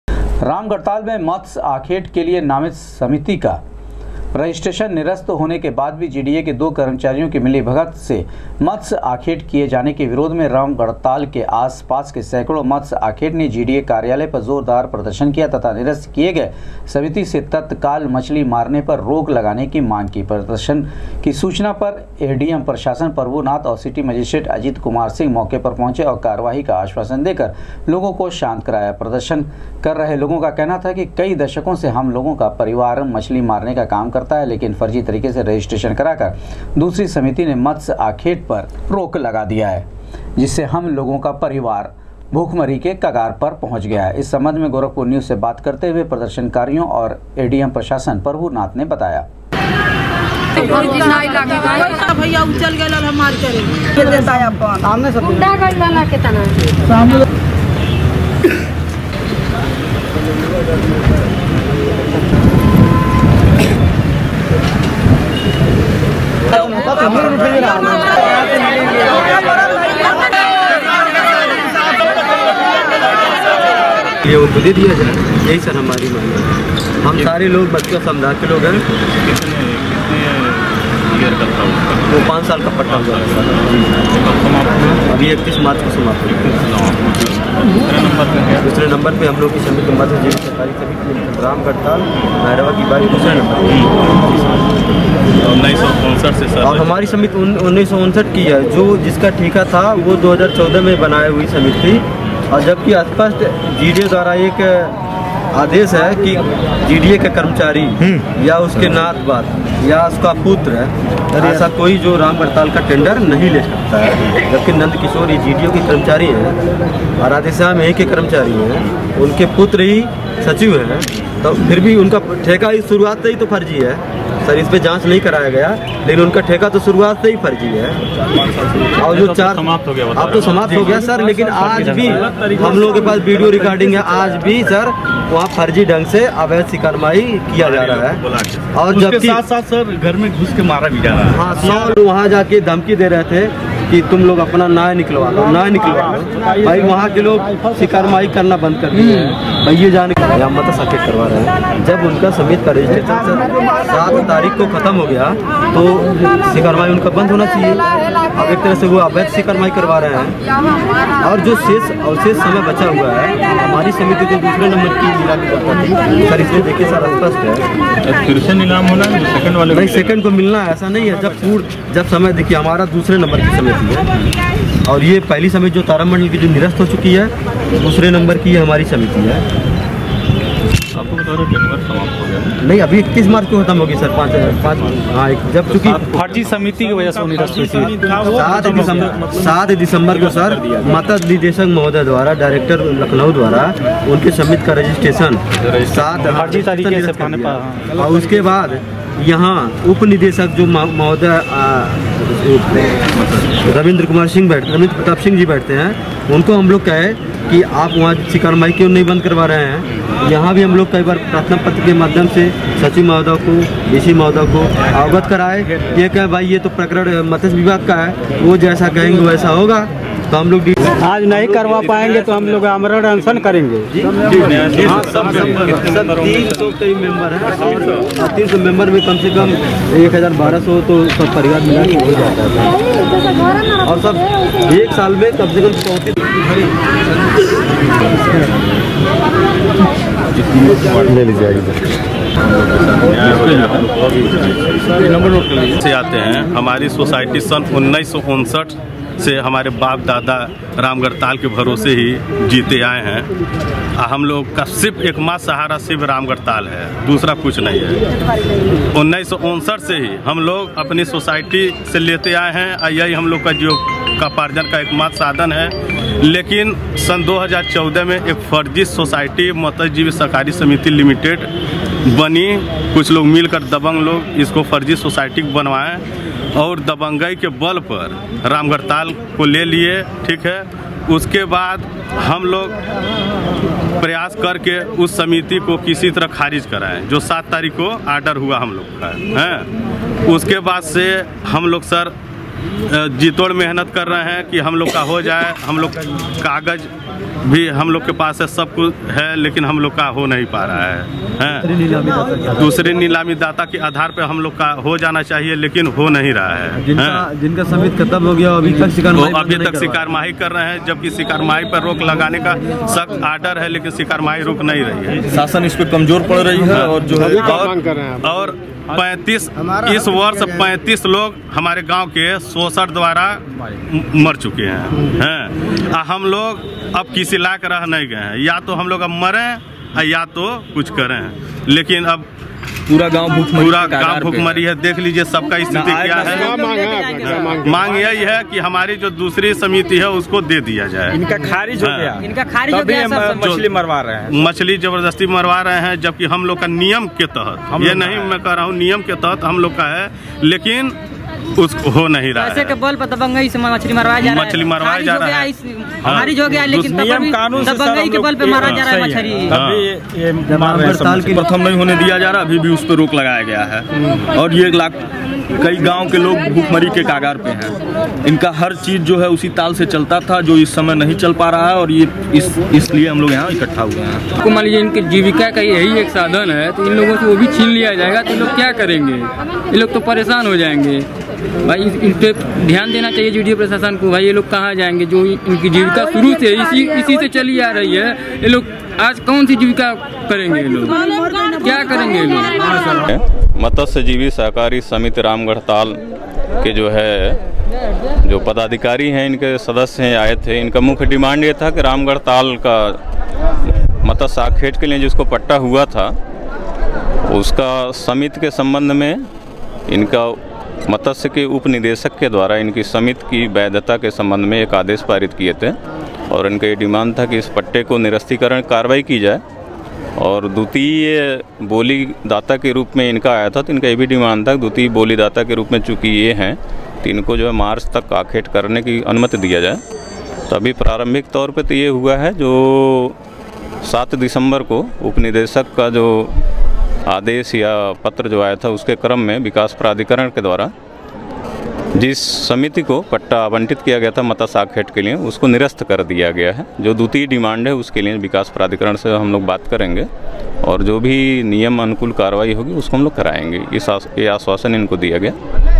रजिस्ट्रेशन निरस्त होने के बाद भी रामगढ़ ताल में मत्स्य आखेट करने का आरोप लगाते हुए स्थानीय
रजिस्ट्रेशन निरस्त होने के बाद भी रामगढ़ ताल में  मत्स्य आखेट करने का आरोप लगाते हुए स्थानीय नागरिकों ने किया प्रदर्शन, मौके पर पहुॅचे एडीएम प्रशासन ने कहा नहीं होगी नाइंसाफी